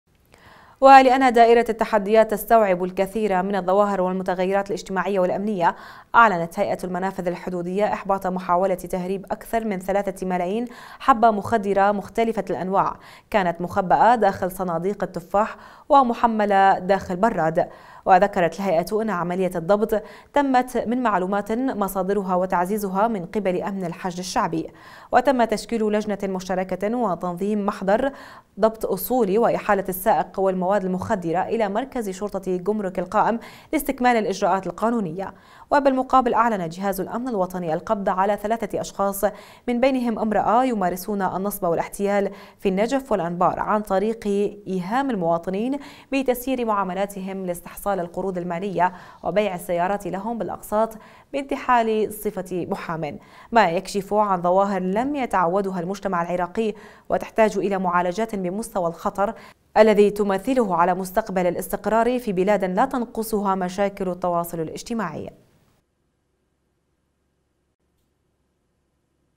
إحباط محاولة تهريب أكثر من ثلاثة ملايين حبة مخدرة في منفذ القائم
ولأن دائرة التحديات تستوعب الكثير من الظواهر والمتغيرات الاجتماعية والأمنية اعلنت هيئة المنافذ الحدودوية إحباط محاولة تهريب أكثر من ثلاثة ملايين حبة مخدرة مختلفة الانواع كانت مخبأة داخل صناديق التفاح ومحملة داخل براد، وذكرت الهيئة أن عملية الضبط تمت من معلومات مصادرها وتعزيزها من قبل امن الحش*د الشعبي، وتم تشكيل لجنة مشتركة وتنظيم محضر ضبط أصولي وإحالة السائق والمواد المخد*رة إلى مركز شرطة كمرك القائم لاستكمال الإجراءات القانونية، وبالمقابل أعلن جهاز الأمن الوطني القبض على ثلاثة أشخاص من بينهم إمرأة يمارسون النصب والاحتيال في النجف والأنبار عن طريق إيهام المواطنين بتسيير معاملاتهم لاستحصال القروض المالية وبيع السيارات لهم بالأقساط بانتحال صفة محامٍ، ما يكشف عن ظواهر لم يتعودها المجتمع العراقي وتحتاج الى معالجات بمستوى الخطر الذي تمثله على مستقبل الاستقرار في بلاد لا تتنقصها مشاكل التواصل الاجتماعي.